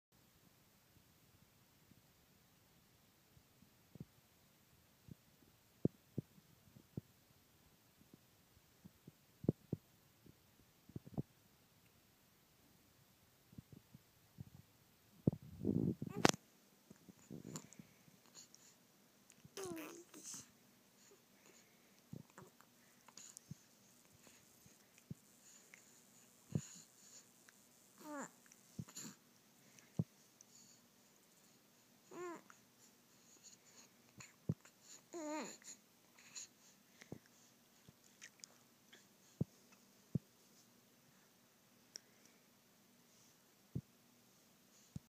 Happy baby boy - 7 weeks
Jackson smiling at 7 weeks old